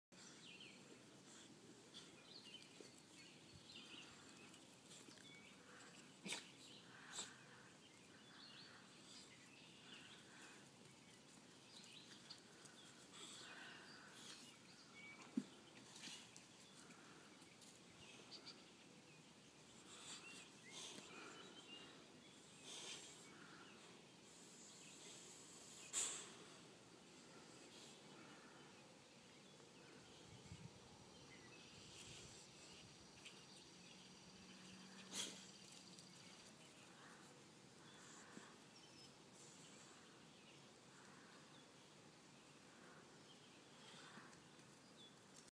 Robin Life
a momma robin nested in the perfect tree for me to keep an eye on her got this amazing video! I've only been waiting like two weeks!!!